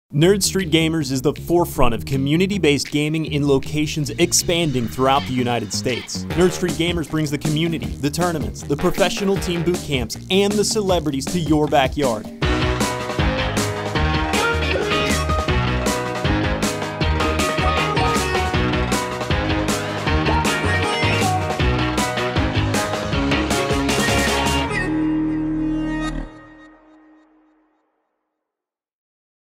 This is N3rdStreetGamers - Amateur Esports Provider
N3rd Street Gamers is an esports network based in Philadelphia and headquartered at Localhost Arena in Northern Liberties that focuses on serving semi-professional, amateur, and developing competitive gamers. Founded in 2011 by entrepreneurs John Fazio and Rob Hilsky in the Jarvus Innovations Incubator, N3rd Street Gamers promotes greater accessibility to the esports industry by not only increasing the amount of in-person competitions, but also by running them on high quality equipment at affordable prices. To accomplish this, N3rd Street Gamers empowers competitive gaming communities with resources to grow and a national system for tracking player progression. Tying together the communities under a unified regional framework in 2018, N3rd Street Gamers launched the NSG Eastern Conference Championship with events spanning three cities and multiple games. Backed by SeventySix Capital, a premier sports tech venture capital firm led by MLB World Series Champion Ryan Howard, N3rd Street Gamers is laying a national framework for esports talent development.